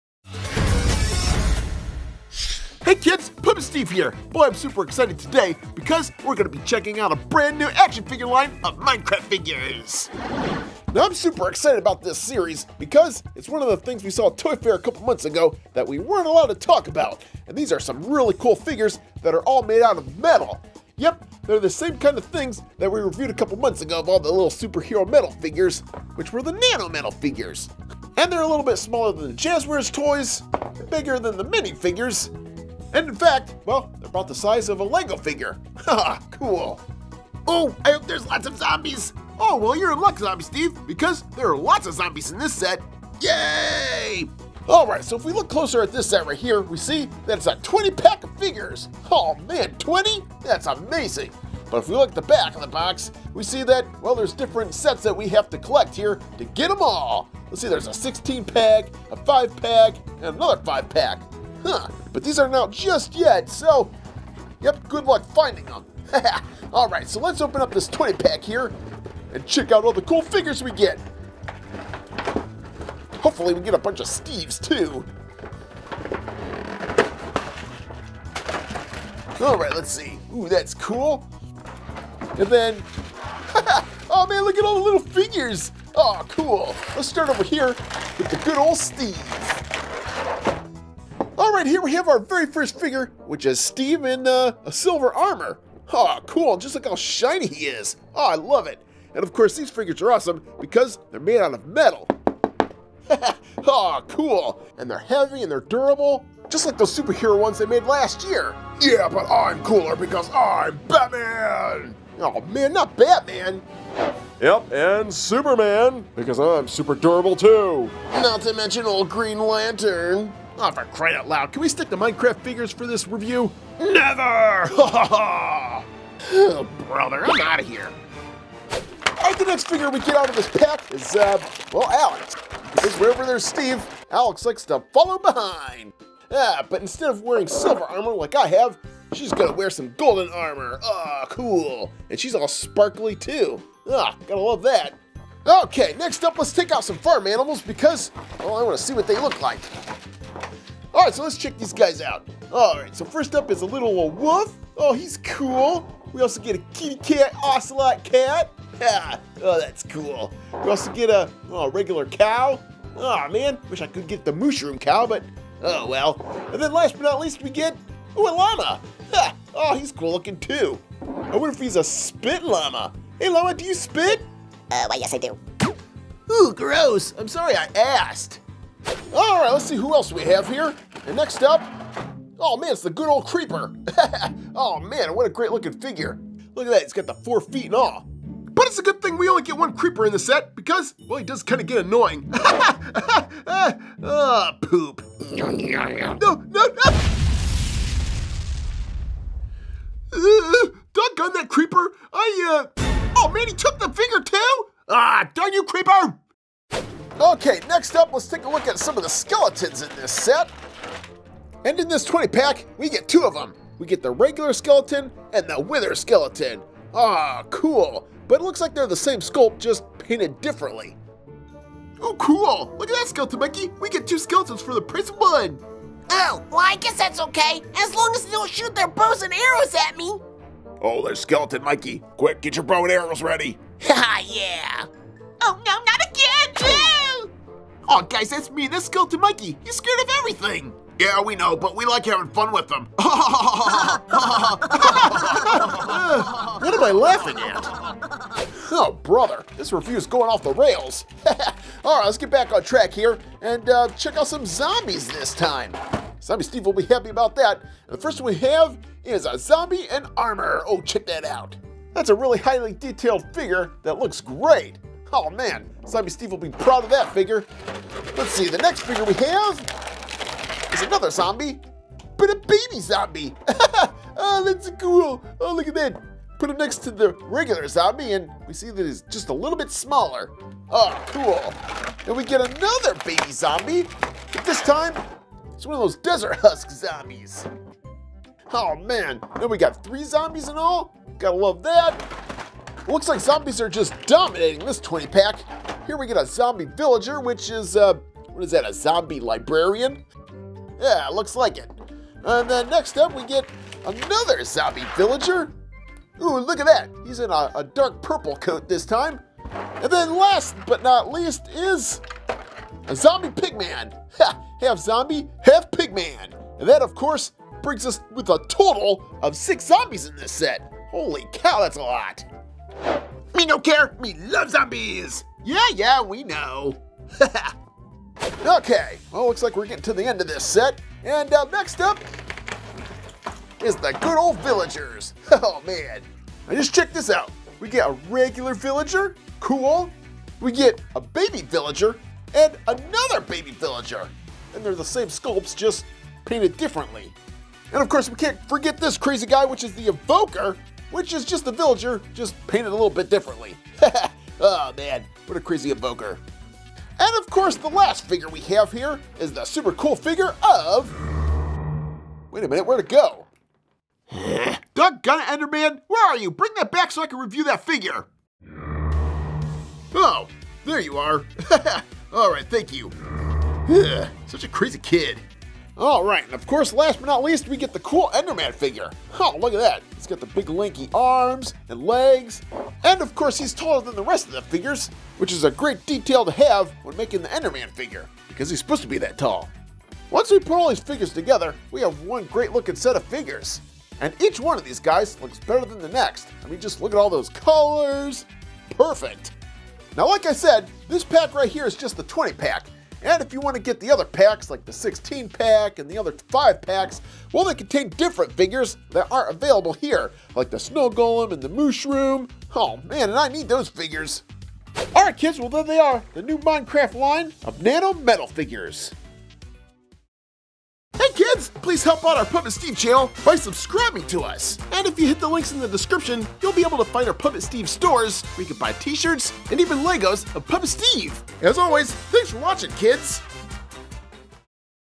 MINECRAFT NEW Nano Metalfigs TOYS! Unboxing Overworld Action Figures
Checking out the brand new toy line of Minecraft Mini NANO Metalfigs Action Figures! We are Unboxing these awesome JADA Toys Overworld PlaySet figs!

Welcome to the Puppet Steve show, home of popular toy unboxings. We mix Toy Unboxings with stop motion, animations, stories, and guest hosts!